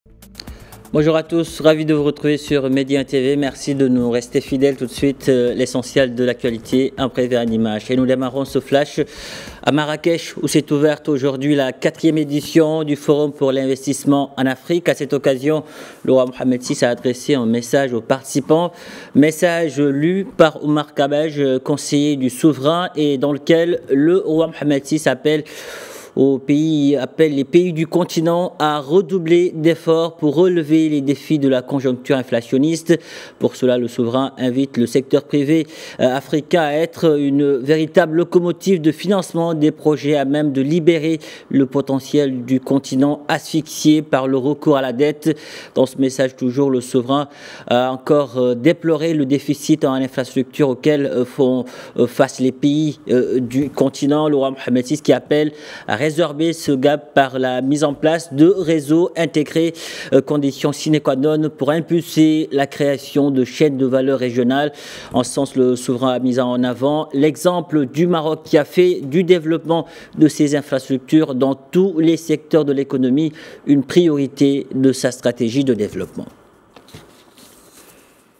Forum pour l´investissement en Afrique : SM le Roi Mohammed VI adresse un message aux participants
SM le Roi adresse un message aux participants à la 4ème édition du Forum pour l'investissement en Afrique